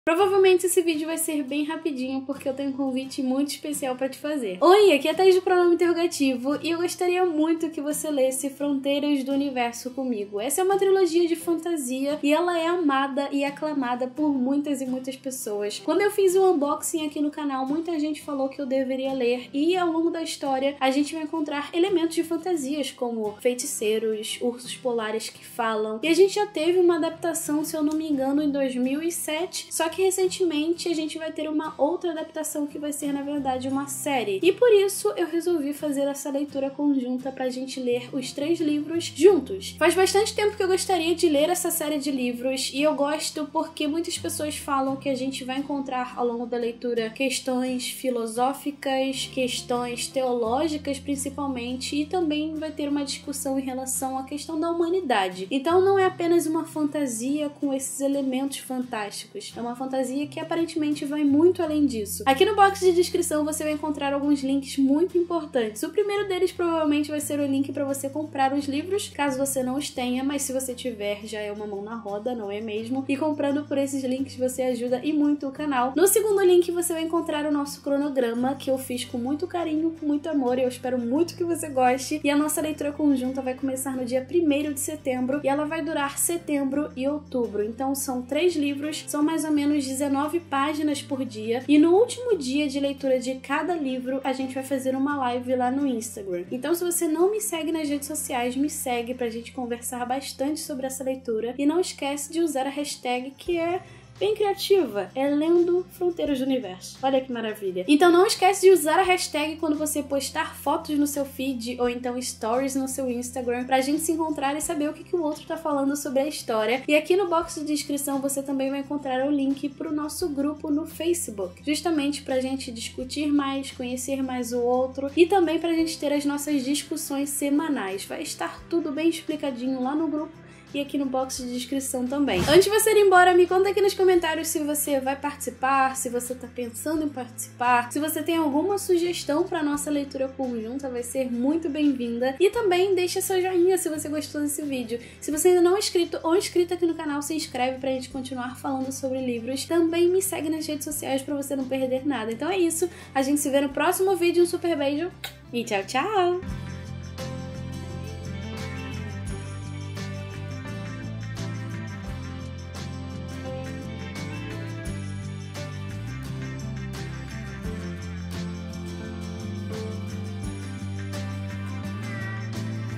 FRONTEIRAS DO UNIVERSO: VAMOS LER? 📚 | Pronome Interrogativo
Fronteiras do Universo é uma trilogia que foi adaptada um vez e agora teremos uma nova adaptação. Vamos ler essa série de livros juntos?

📌 Use a LendoFronteirasdoUniverso